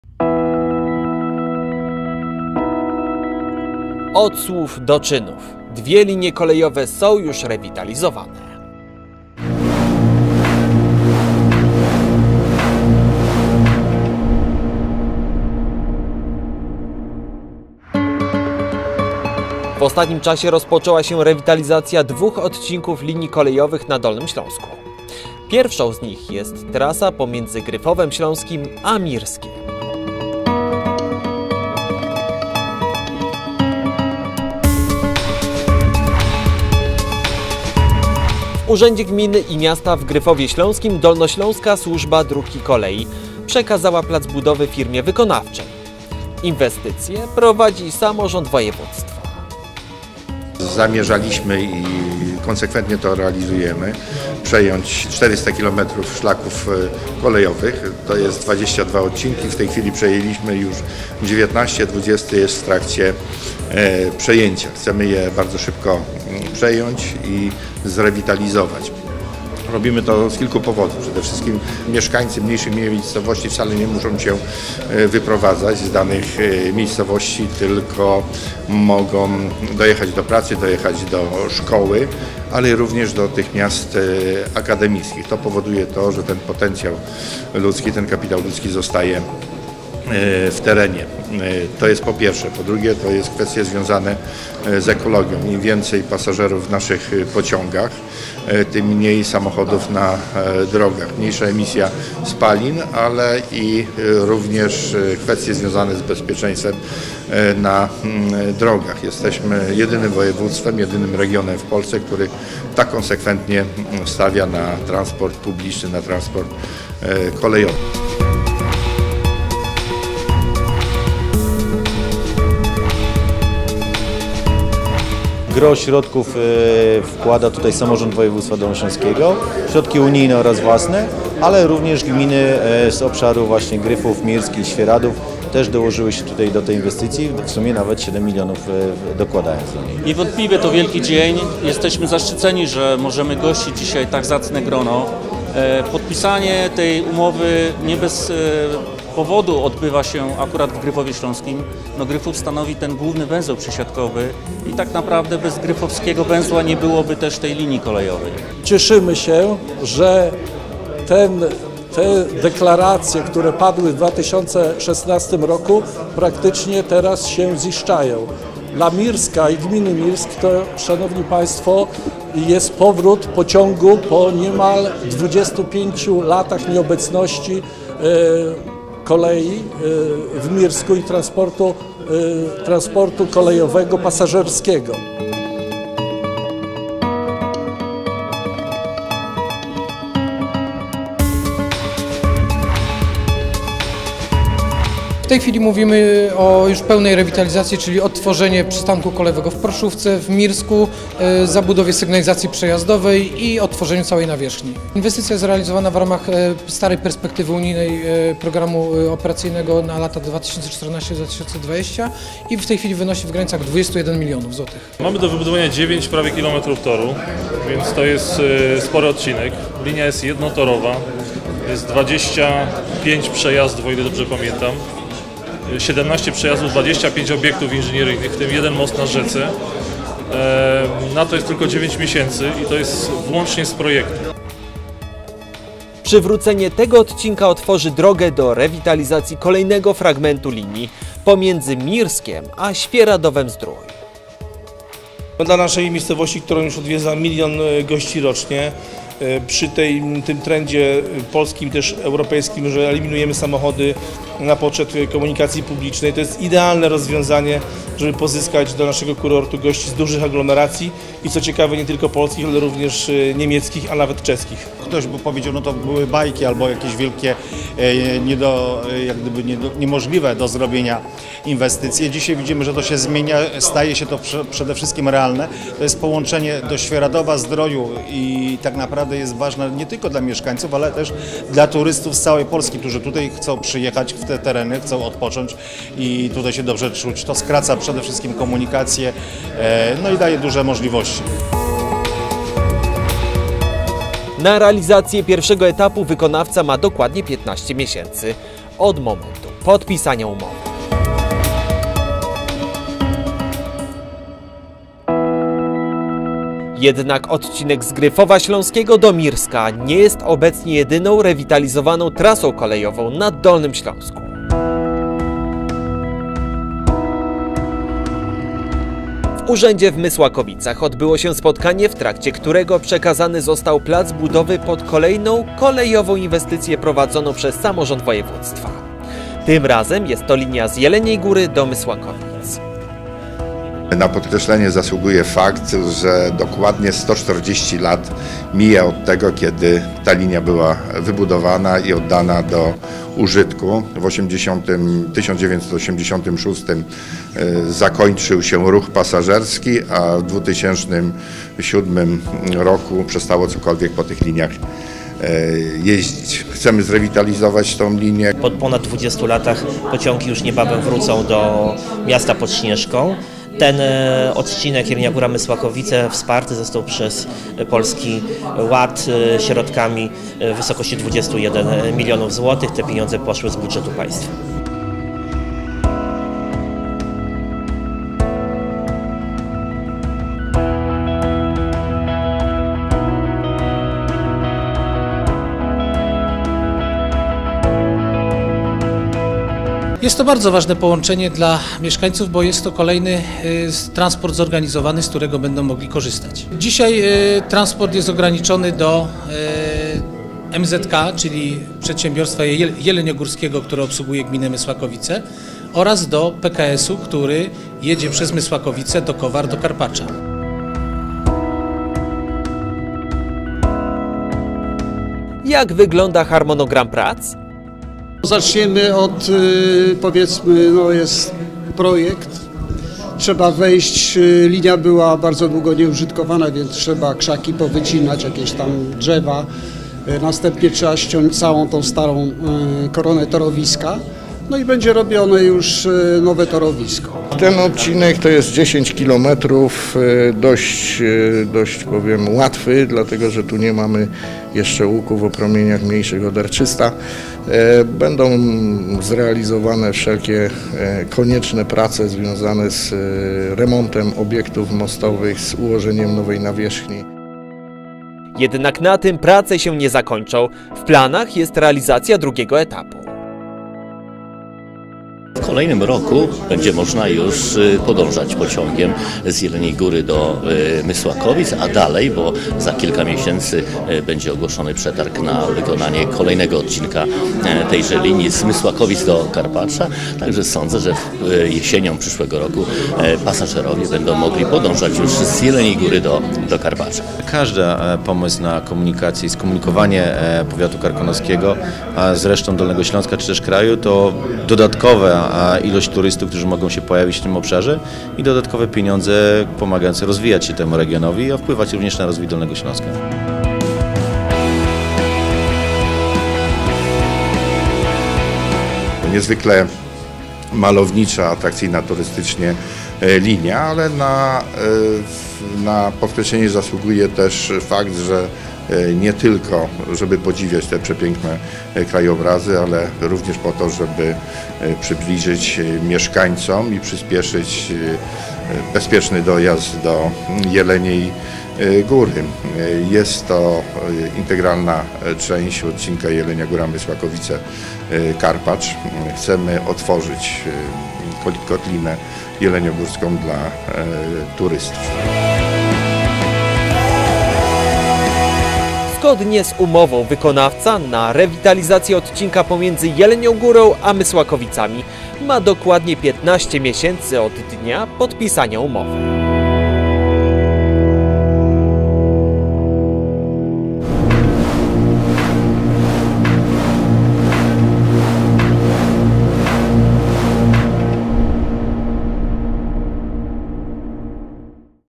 Dolny Śląsk Górą - Ruszyła rewitalizacja dwóch linii kolejowych
W ostatnim czasie rozpoczęły się rewitalizacje II odcinków linii kolejowych na Dolnym Śląsku. Pierwszą z nich jest trasa pomiędzy Gryfowem Śląskim a Mirskiem, a druga pomiędzy Jelenią Górą a Mysłakowicami.